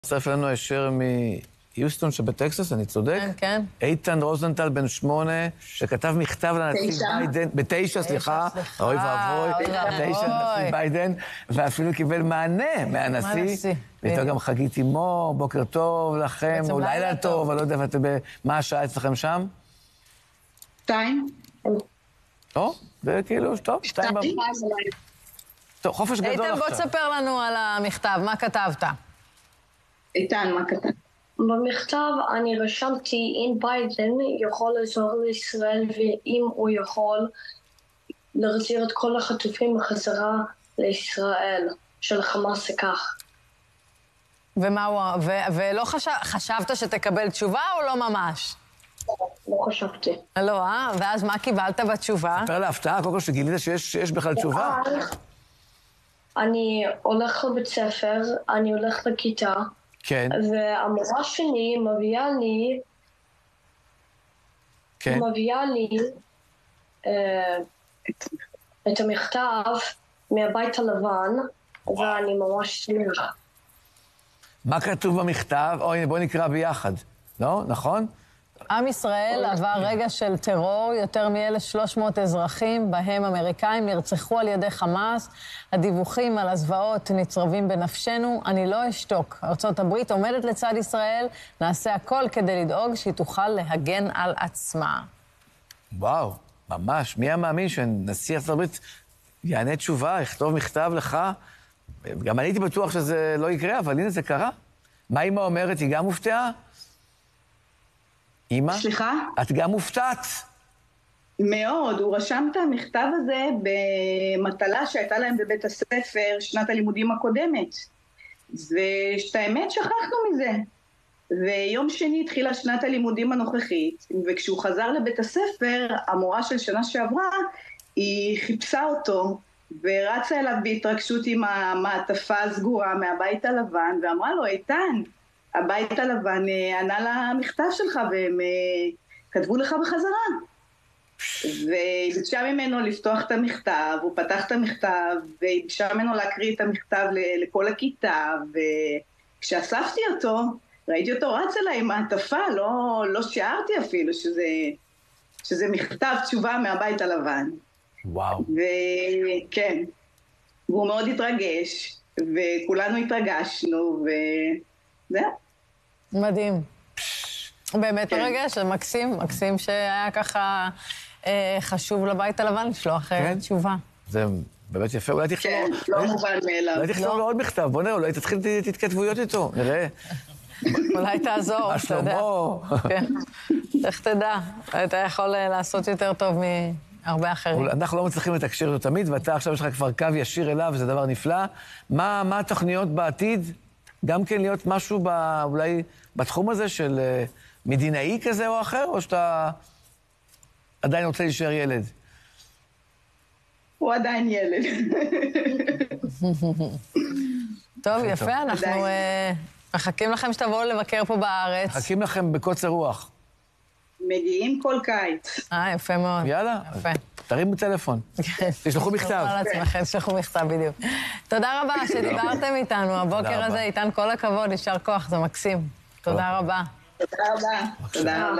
"ארה"ב עומדת לצד ישראל": הילד שקיבל מכתב מהנשיא ביידן בריאיון מיוחד
ערוץארבעשרה